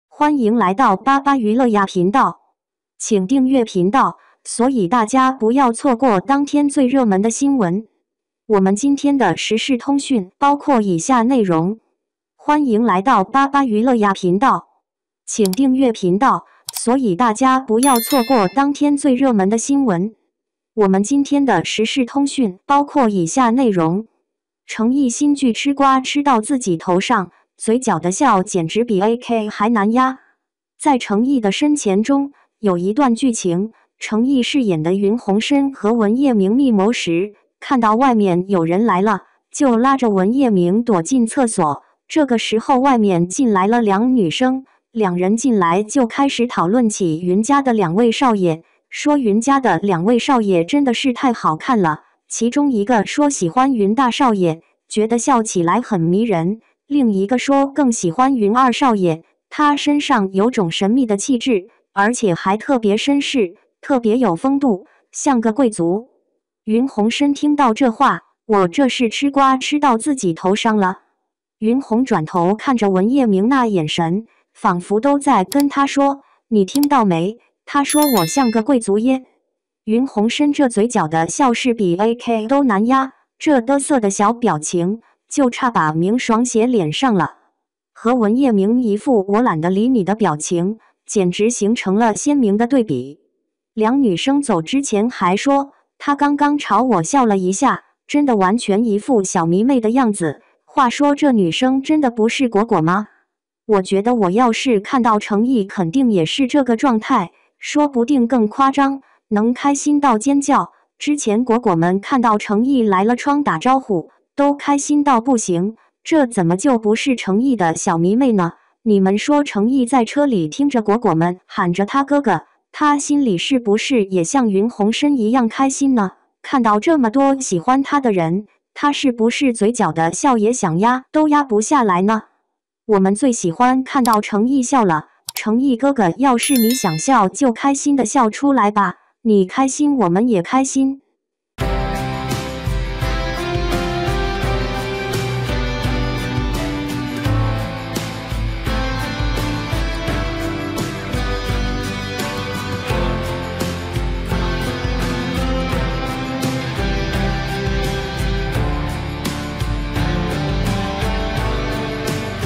成毅新剧吃瓜吃到自己头上，嘴角的笑简直比AK还难压！
成毅新剧吃瓜吃到自己头上，嘴角的笑简直比AK还难压！ | 八八娱乐呀